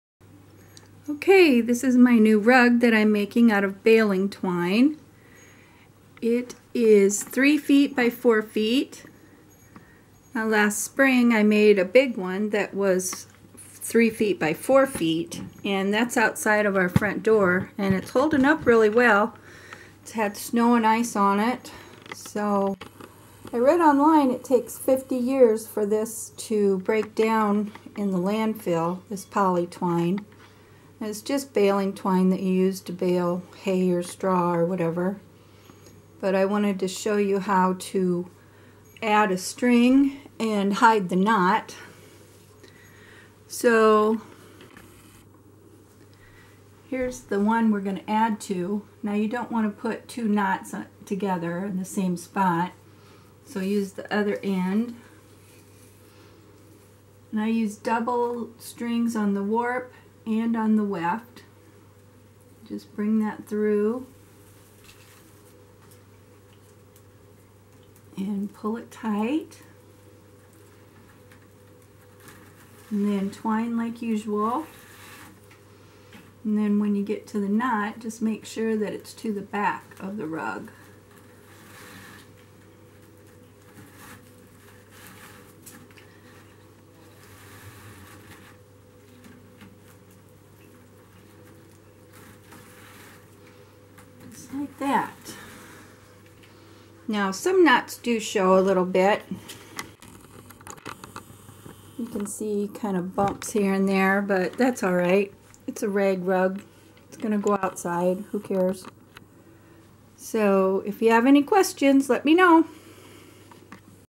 Baling twine rug
This is a twined rug made from poly baling twine. Twined woven rug